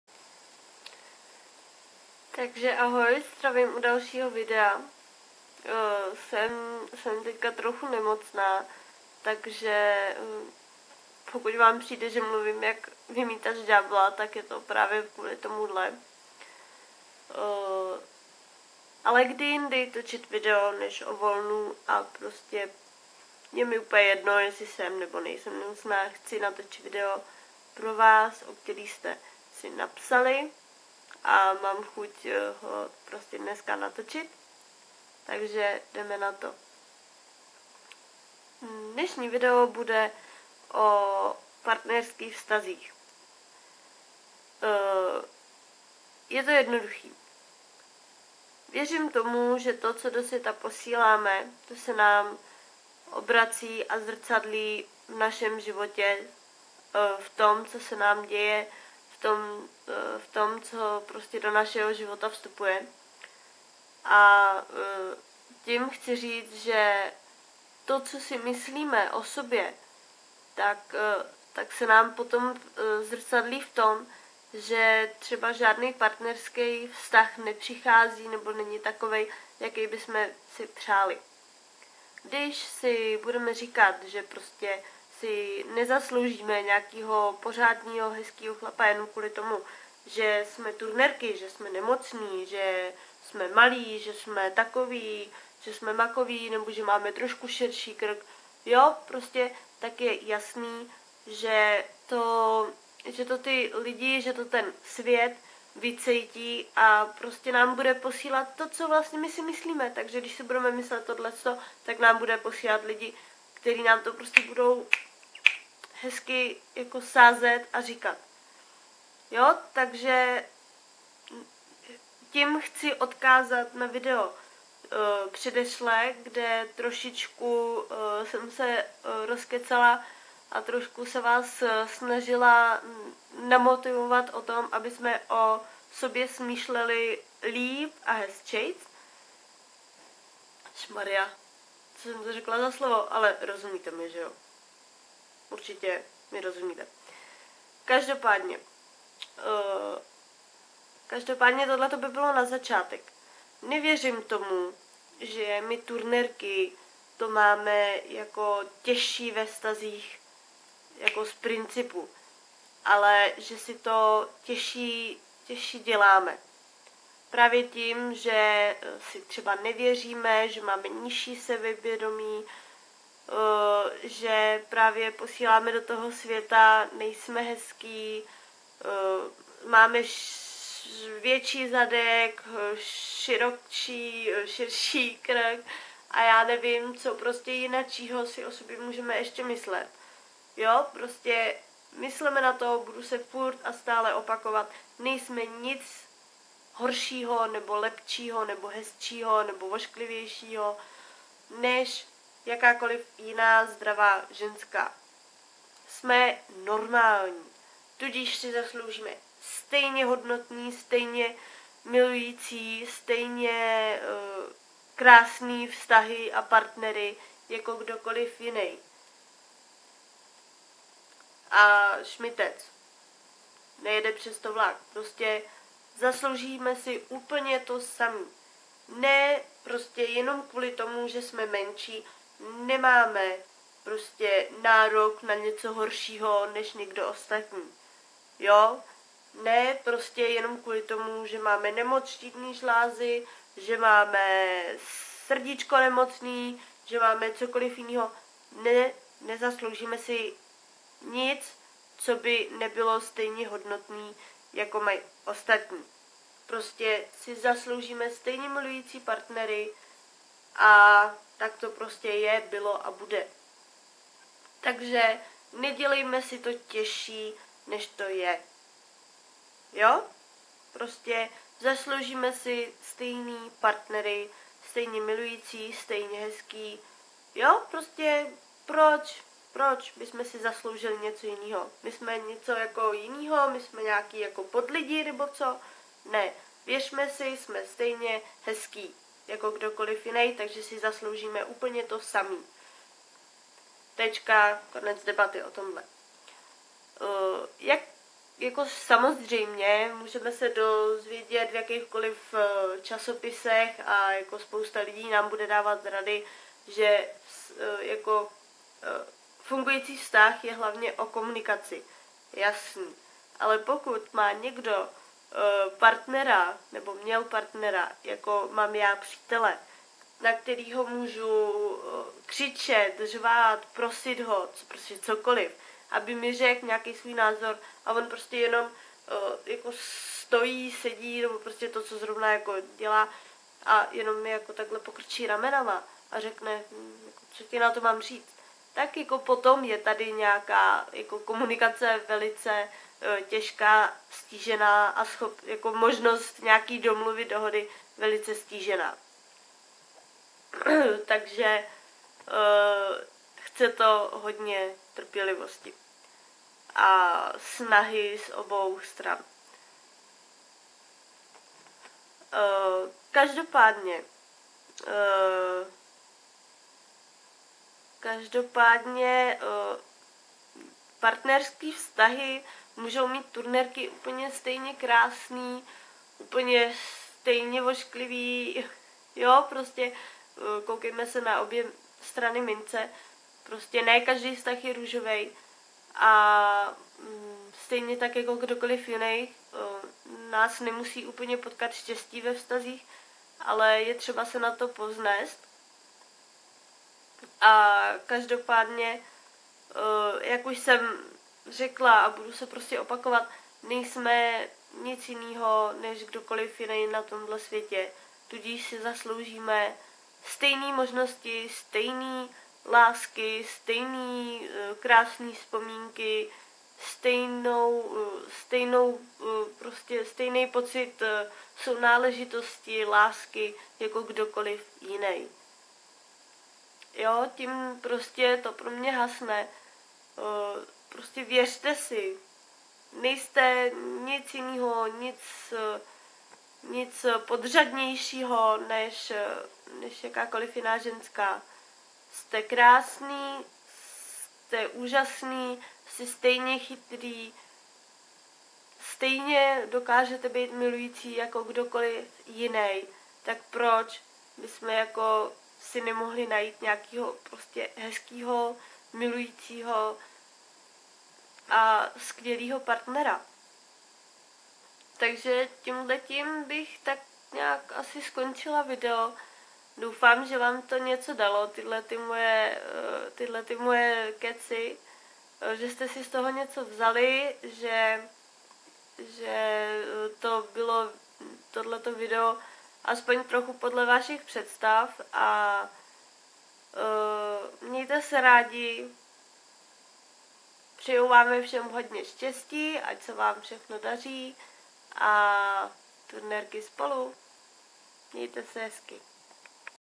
Vztahy a TS/ nedělejme si to těžší, věřme si....
Nejsme žádný podlidi, zasloužíme si to samý co ostatní a to platí i v lásce.
Užijte si páteční video a mějte se rády, mějte se krásně a čau.
Jo a nezapomeňte mě sledovat na sociálních sítích, sdílejte lajkujte atd..
Turnerky spolu